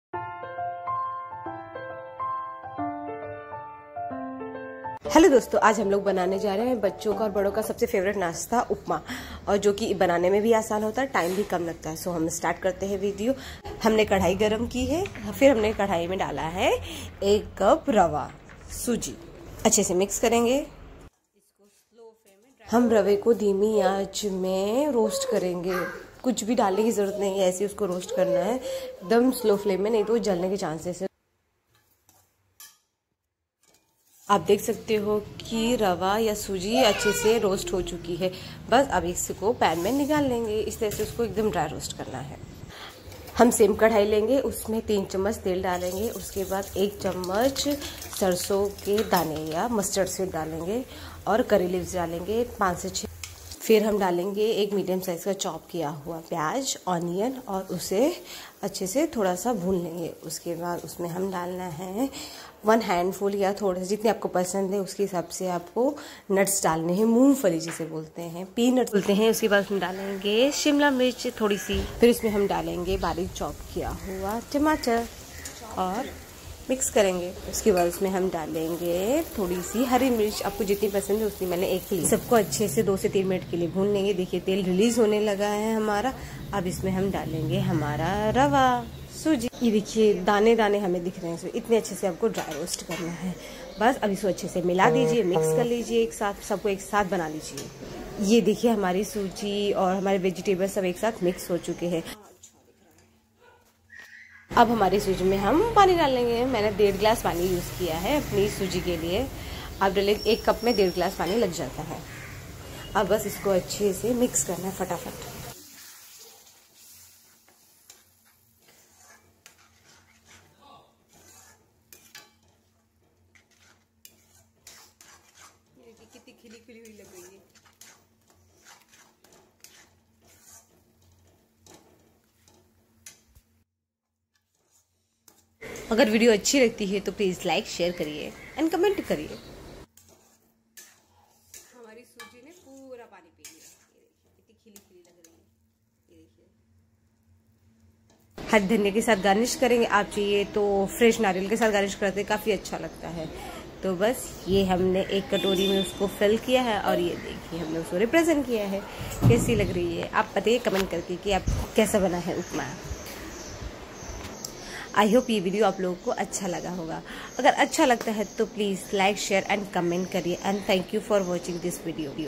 Soft fluffy upma~indian breakfast~easy उपमा recipe ~rava upma~suji upma विधि ~ ~Healthy nutritious
उपमा बनाने की आसान विधि ~बच्चों का पंसदीदा नास्ता और Healthy ~

Ingredients
Rava - 1 cup (semolina)
Peanuts - 1 handful
Capsicum - 1 small size
Tomato - 1small size